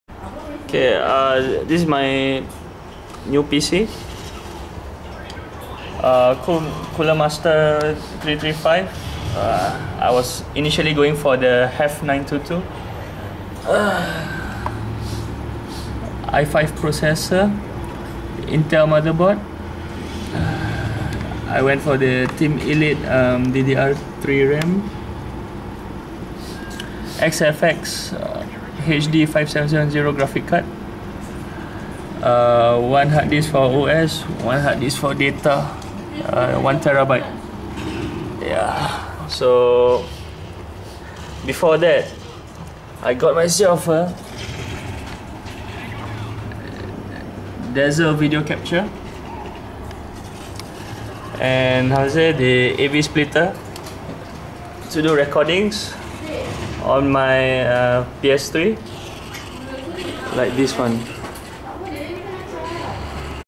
Media DIY Machine
Machine I just built yesterday.

Casing: Cooler Master Elite 335
PSU: Cooler Master GX 650W
MB: Intel DH55HC
CPU: Intel i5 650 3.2GHz 4MB Cache
RAM: Team Elite PC1333 4GB (2GB x 2 kit)
GFX: XFX Radeon HD 5770 1GB DDR5